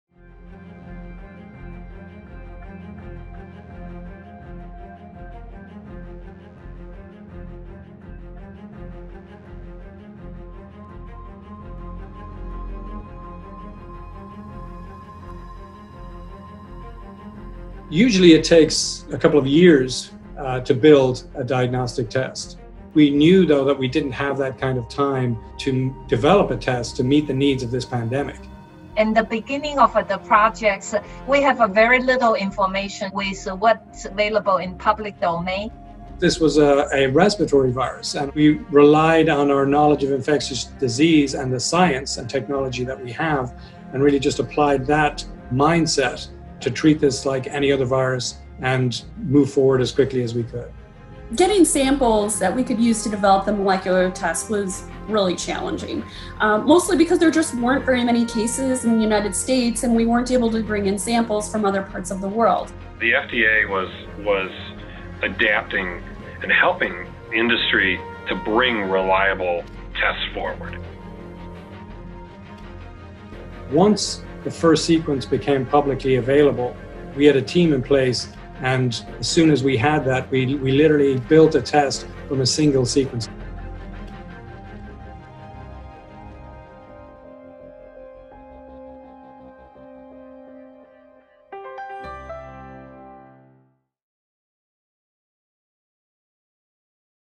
Testing Our Limits, Episode 2: The Path Forward
It’s soon apparent that meeting daily won’t cut it. The work needs to happen ‘round the clock, 24/7. And even with that dedication from peoples with decades of experience, they’re going to some good luck to break their way too, particularly when it came to acquiring the samples needed to design a test that could identify COVID-19.

“Getting samples that we could use to develop the molecular test was really challenging, mostly because there just weren’t very many cases in the United States and we weren’t able to bring in samples from other parts of the world,” said Mary Rodgers, principal scientist for infectious disease research, Abbott Diagnostics.

Added Gavin Cloherty, head of infectious disease research, Abbott Diagnostics: “Once the first sequence became publicly available, we had a team in place, and as soon as we had that, we literally built a test from a single sequence.”

Learn more about how Abbott’s diagnostics are helping to slow the COVID-19 pandemic